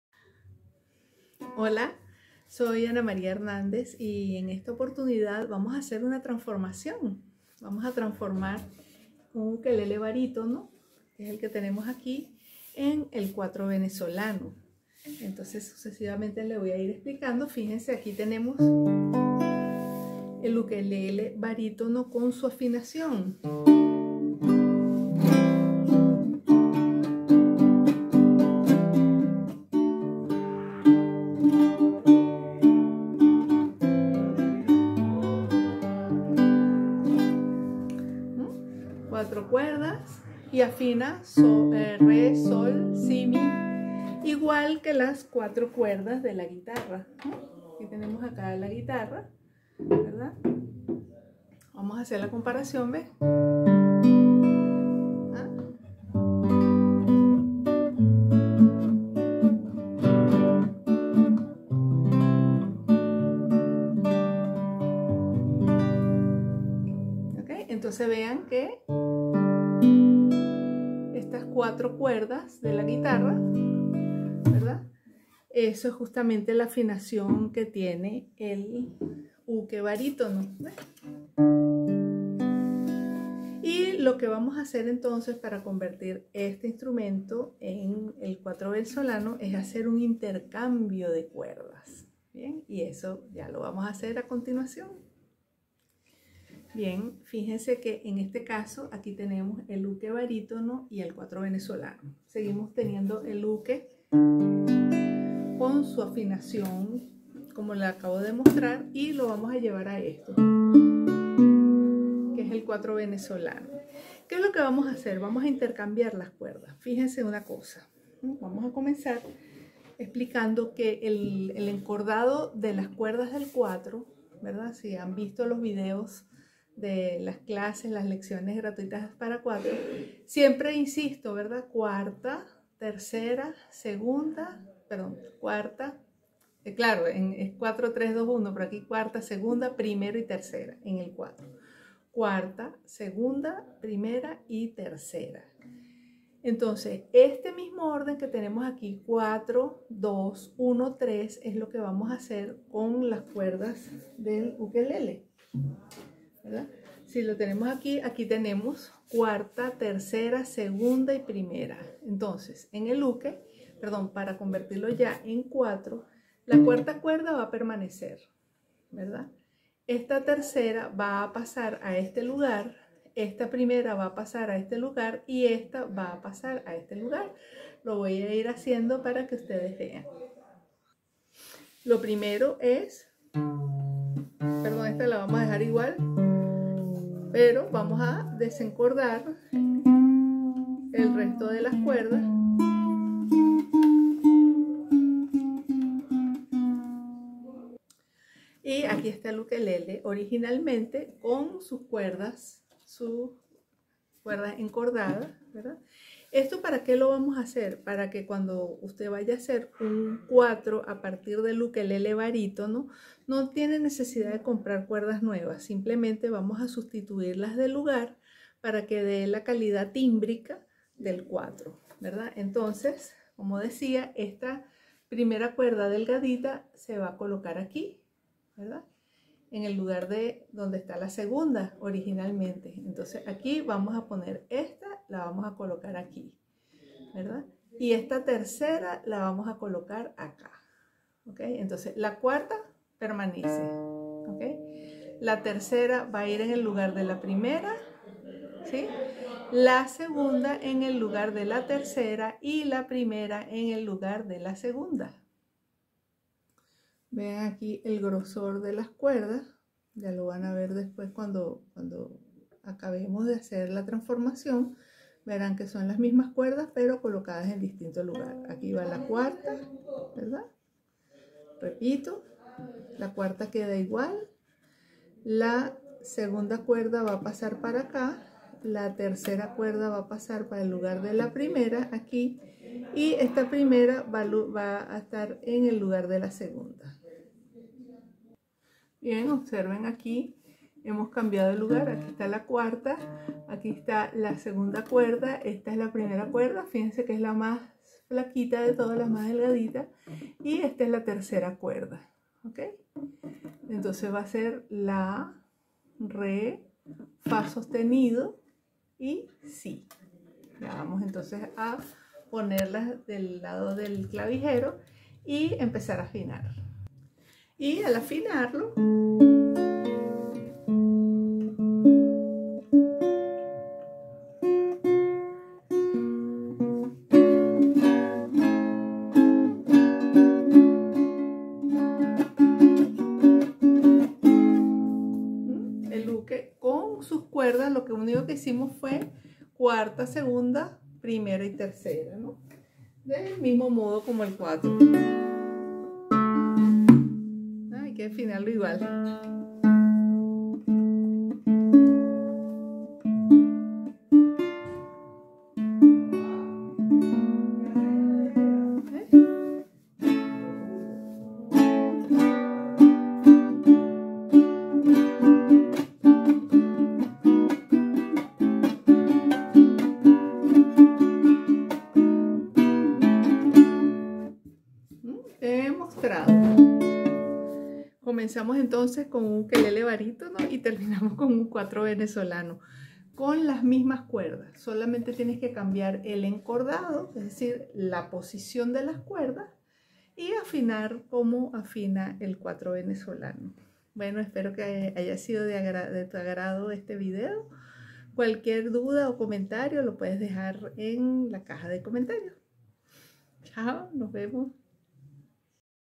CÓMO CONVERTIR UN UKELELE BARÍTONO EN CUATRO VENEZOLANO
Aquí les explico paso a paso cómo cambiar las cuerdas del ukelele barítono para obtener el encordado del cuatro venezolano: si el orden de las cuerdas del ukelele barítono es 4, 3, 2, 1 (de la más gruesa a la más finita), el encordado del cuatro es 4, 2, 1, 3. Por lo tanto, todo lo que hay que hacer es cambiar el orden de las cuerdas, afinar y tocar.
Aquí les dejo el enlace de mi video SEMEJANZAS ENTRE EL UKELELE BARÍTONO Y EL CUATRO VENEZOLANO